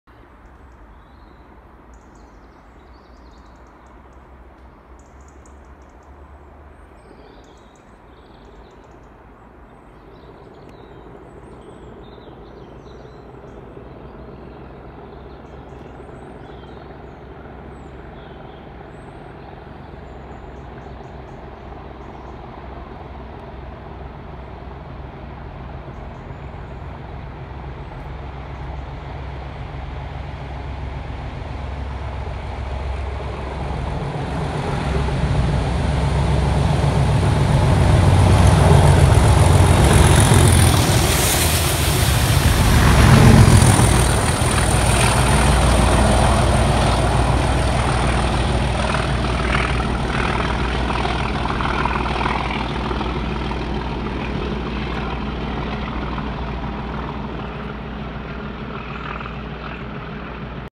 37423&37402 on 15:19 Hull to York Thrall Europa passing Sherburn in Elmet 01/10/2020